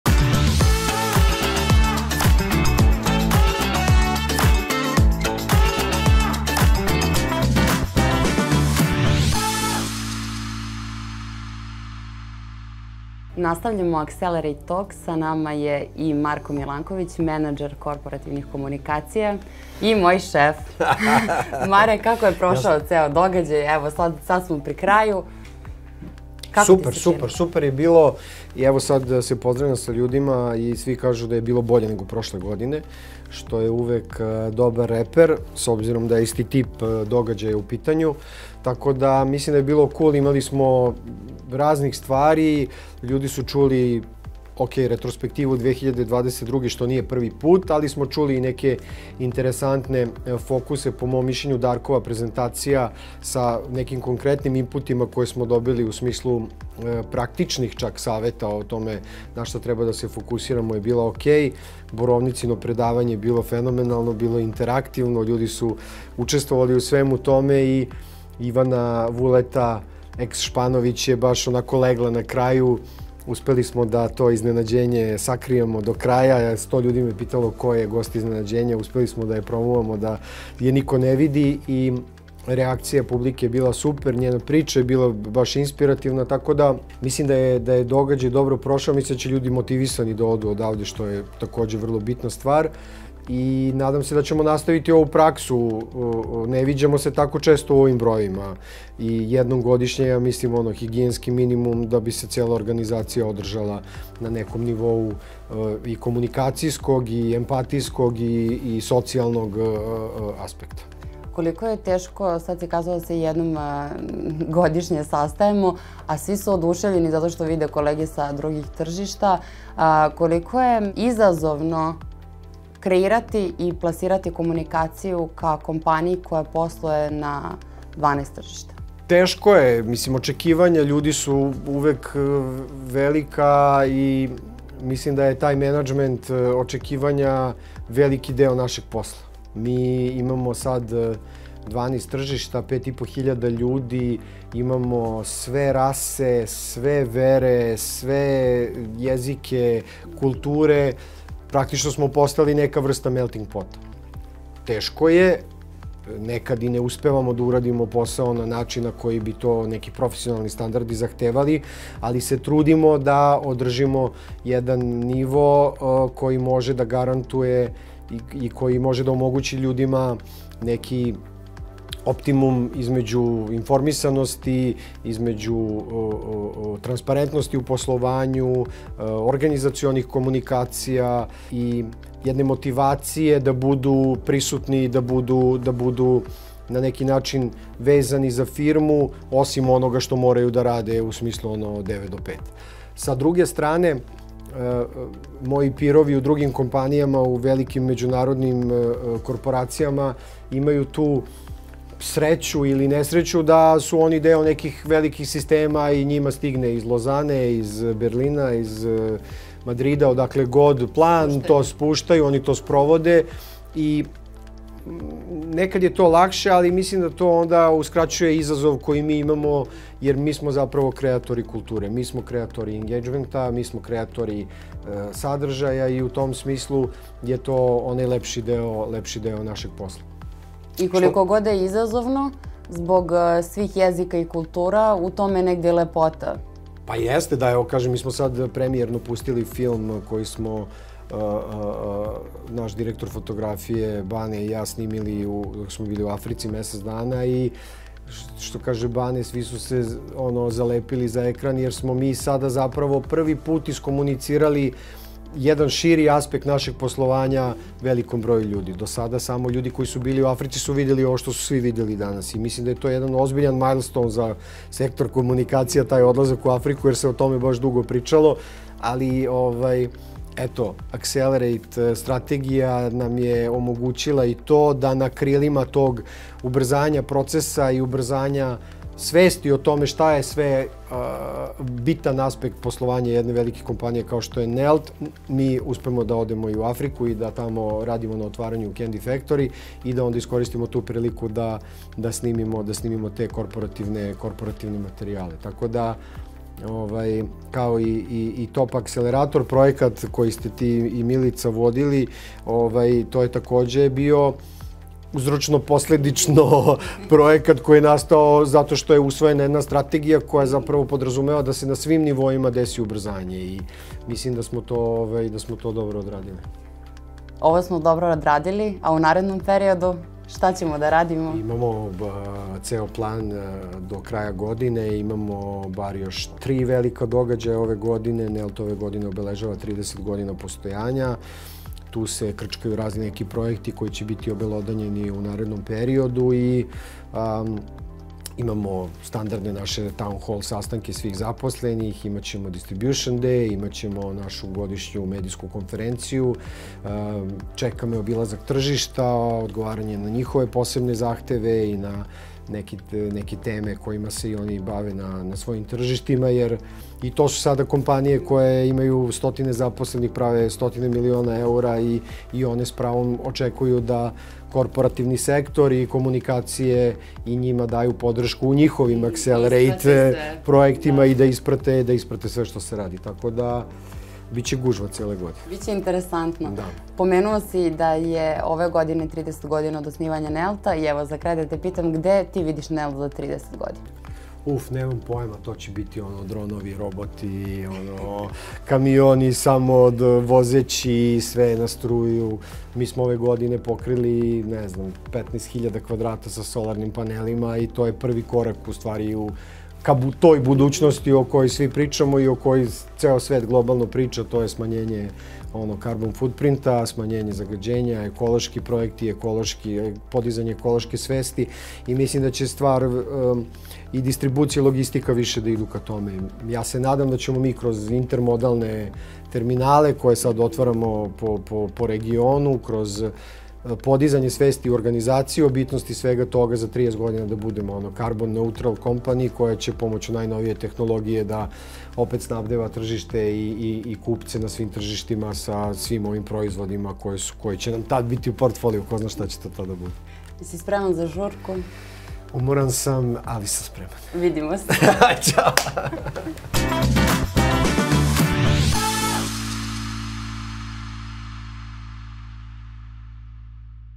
Nelt Talks | Epizoda 8 | Gost: Marko Milanković, Nelt Group
Marko Milanković, menadžer korporativnih komunikacija Nelt Grupe, gost je Divne Maletić u osmoj epizodi emisije „Nelt Talks“.🎙

Podrška:

Kamera: Strahinja Marković, Ivan Todorovski
Snimatelj zvuka: Dejan Čeko
Rasveta: Andrej Vrcelj
Uvodna špica: Srđan Vasiljević
Montaža: Srđan Vasiljević

Zabranjeno je svako kopiranje i preuzimanje u delovima ili celosti. Nelt Grupa je vlasnik materijala i zadržava sva prava na objavljeni sadržaj.